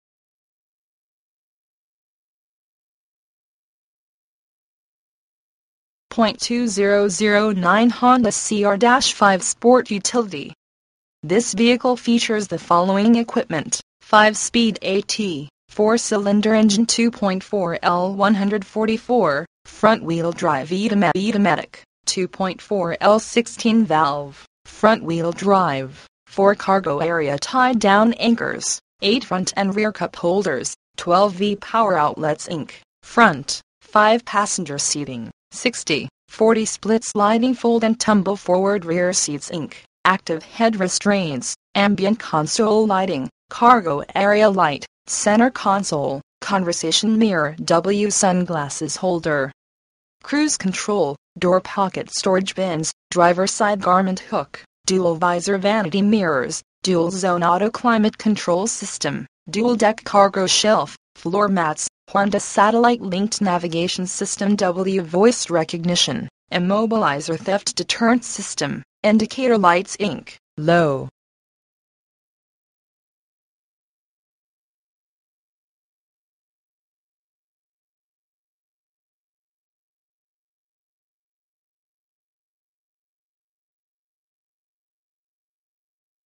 2009 Honda CR-V - Northside Lexus - Houston, TX 77090
Northside Lexus
17925 North Freeway

For Sale: 2009 Honda CR-V
VIN: JHLRE38719C001152
Engine: 4 Cylinder Engine 2.4L/144
Drivetrain: Front Wheel Drive
Transmission: 5-Speed A/T
Mileage: 83,428
Color: Black (exterior) Tan (interior)